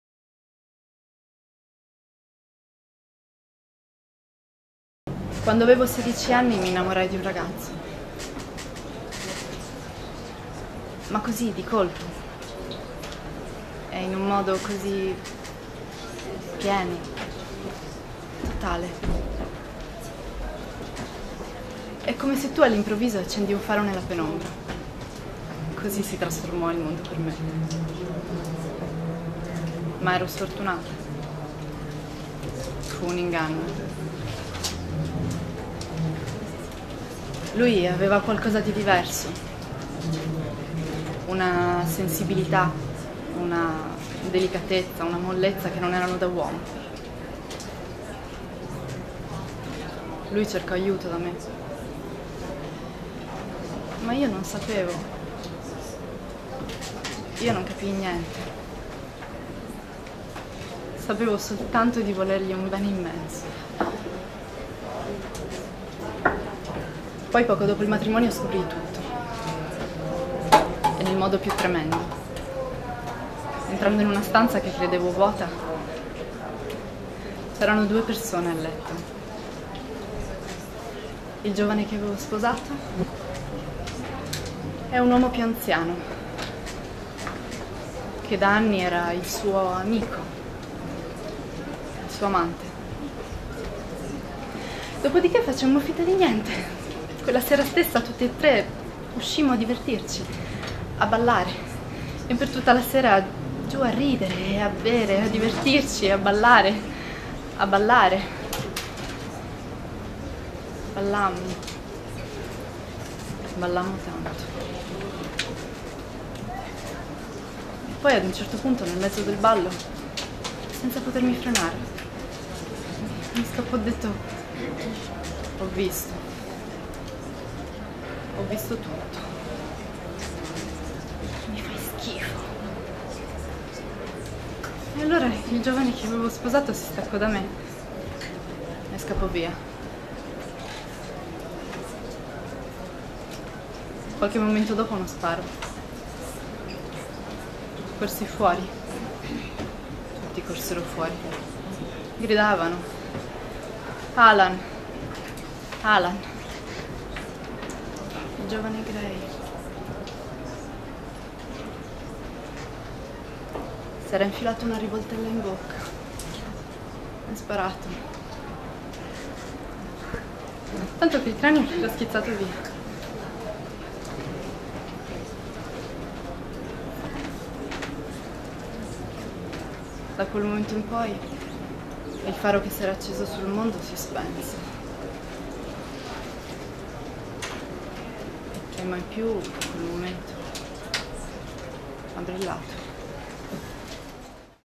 Francesca Aledda, Nove Giorni di Grandi Interpretazioni, 2013, Il Gioco Del Lotto, RB Casting
Francesca Aledda, Provino, al Festival Internazionale del Film di Roma, Nove Giorni di Grandi Interpretazioni, 2013, promosso da Il Gioco Del Lotto, e Presentato da RB Casting